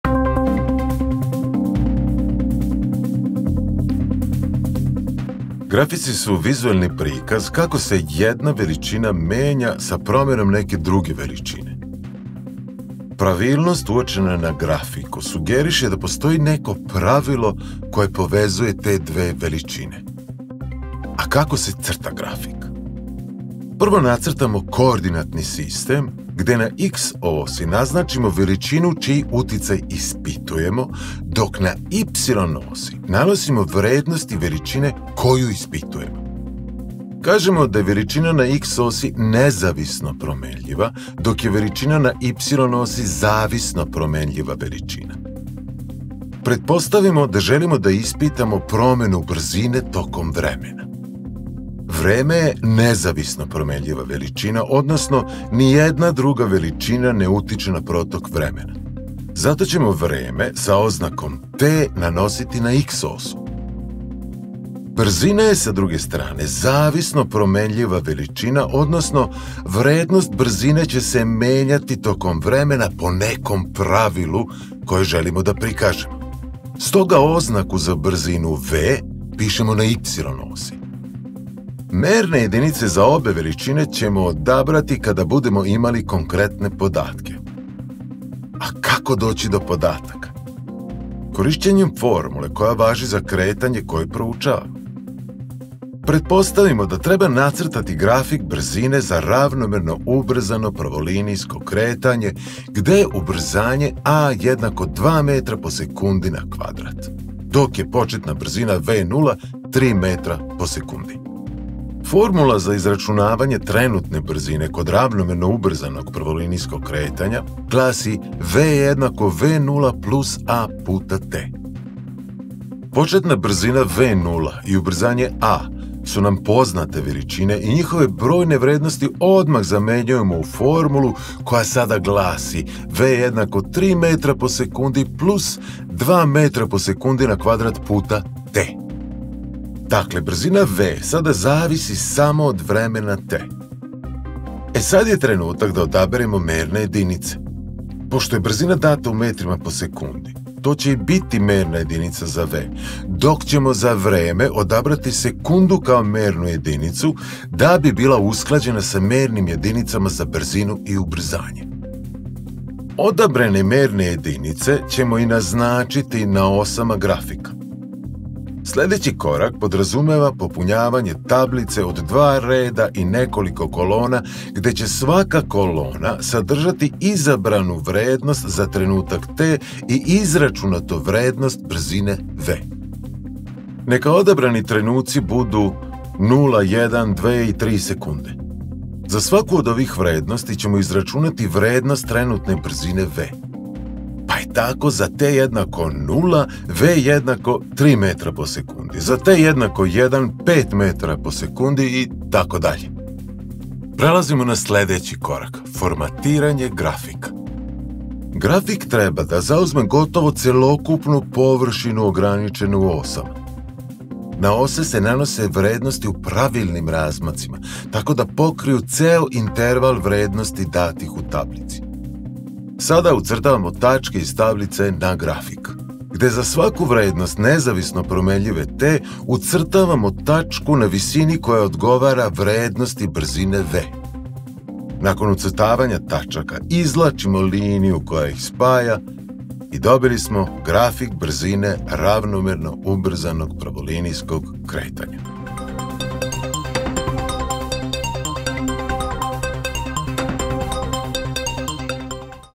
Grafik brzine
Crtanje grafika brzine za ravnomerno ubrzano pravolinijsko kretanje. Promena brzine tokom vremena.